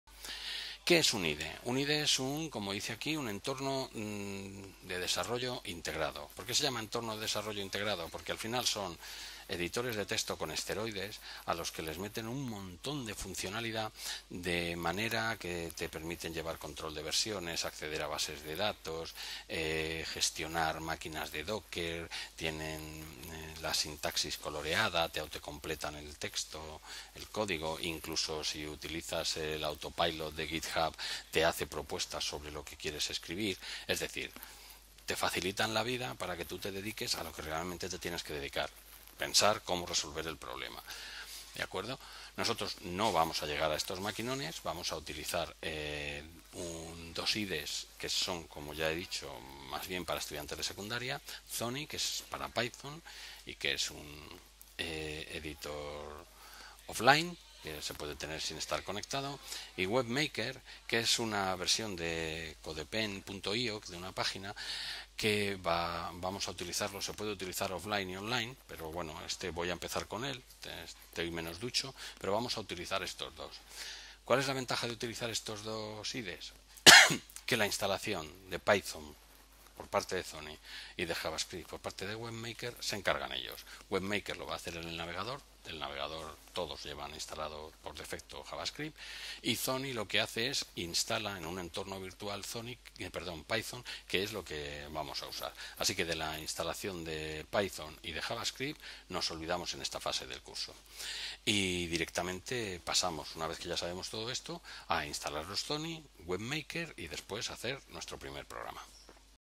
Curso de Programación | ¿Qué es un IDE en PROGRAMACIÓN? (Entorno de Desarrollo Integrado)
¿Te has quedado con ganas de más? Inscríbete en nuestro Bootcamp Full Stack JR.

0:00 Intro
0:03 ¿Qué es un IDE en Programación?
0:08 ¿Por qué se llama Entorno de desarrollo integrado?
0:47 Ejemplos de IDE's